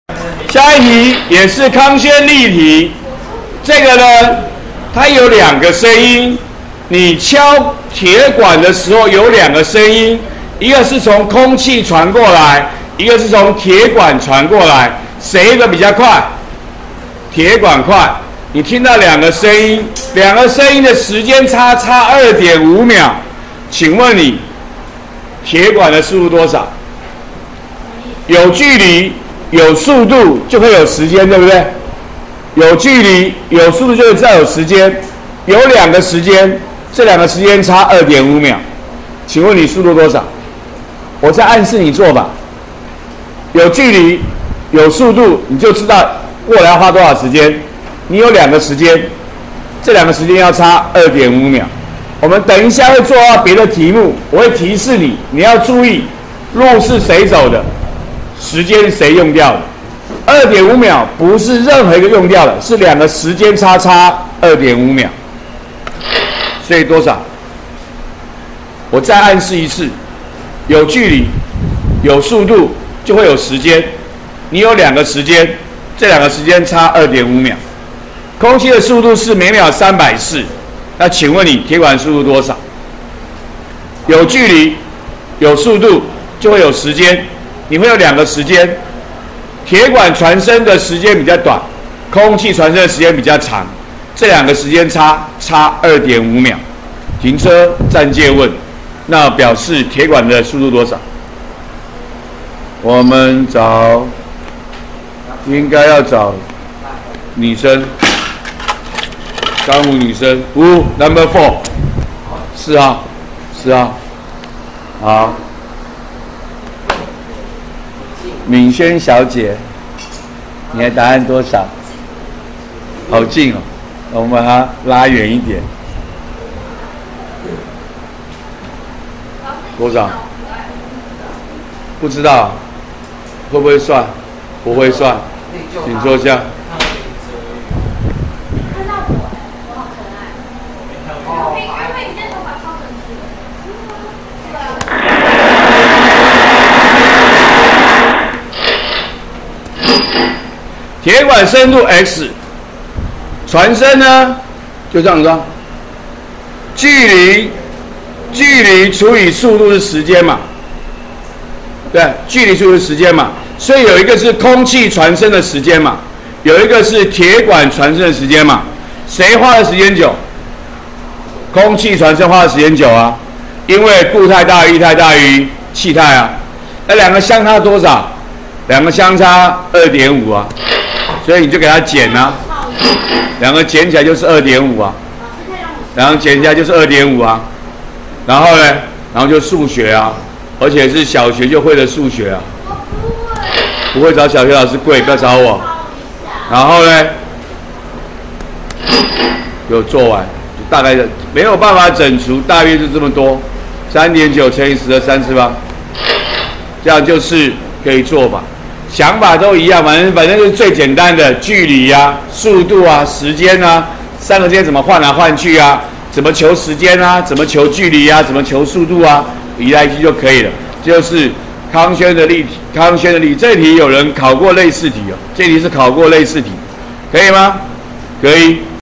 104學年國二理化＿聲音08：練習10
練習10是敲擊已知長度鐵管，聽到鐵管及空氣傳來聲音，相差2.5秒，已知空氣傳聲速度，問鐵管傳聲速度。